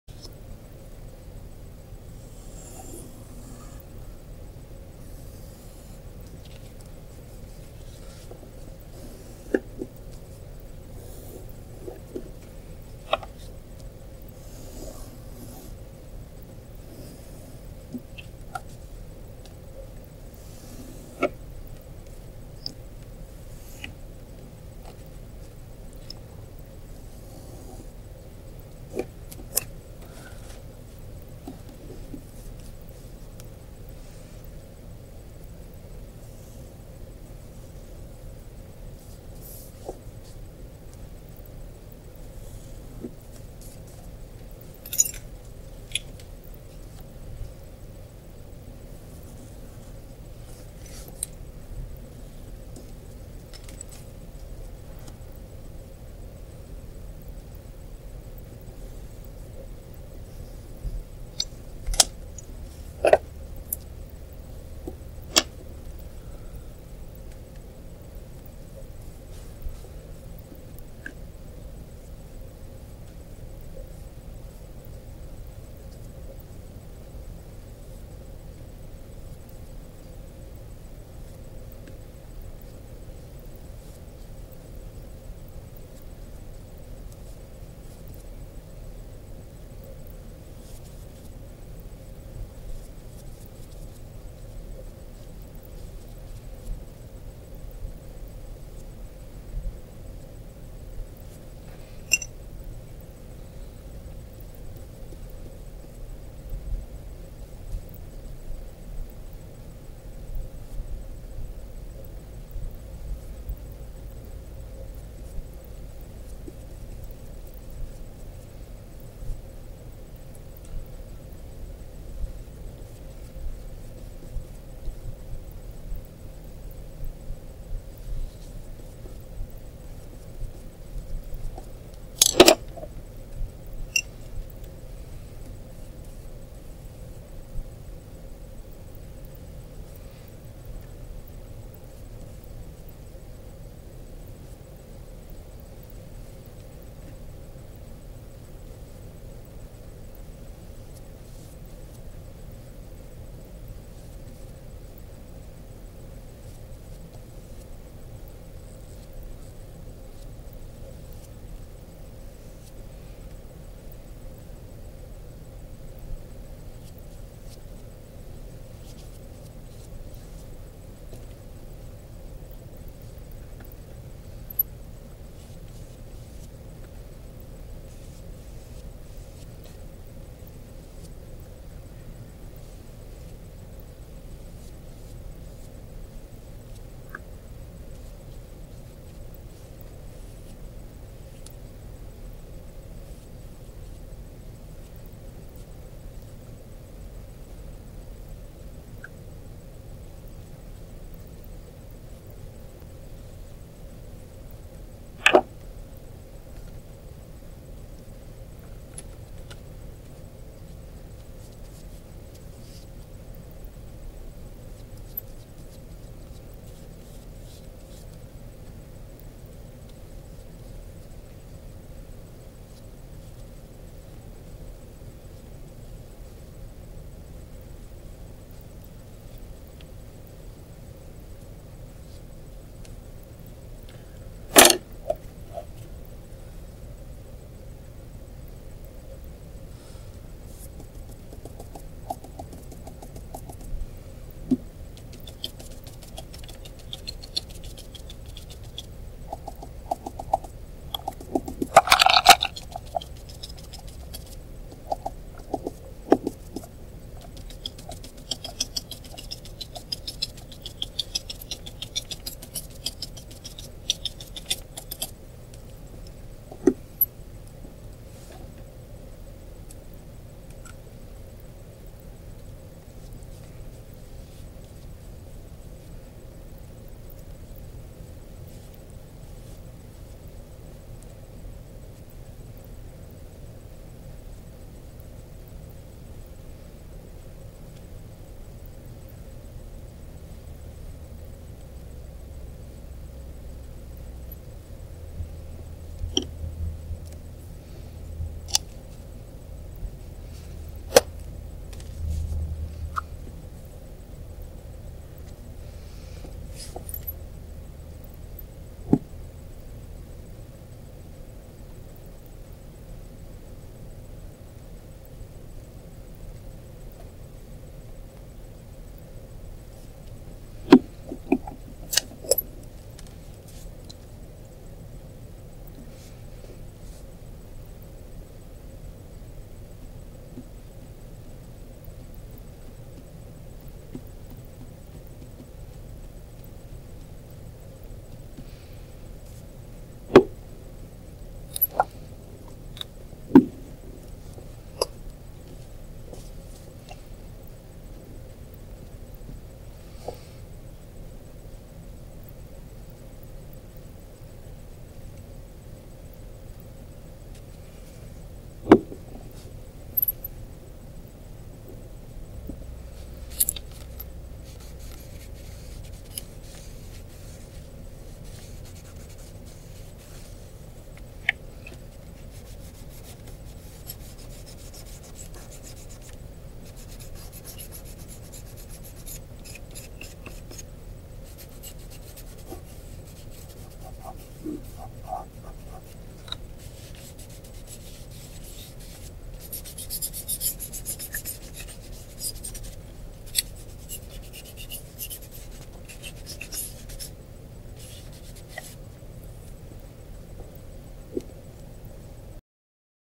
Wayne Theibaud Donut demonstration
Here is a short demo of how my 4th graders created their Donut paintings. Materials used include tempera paint, charcoal, and a 12x18in sheet of colored construction paper.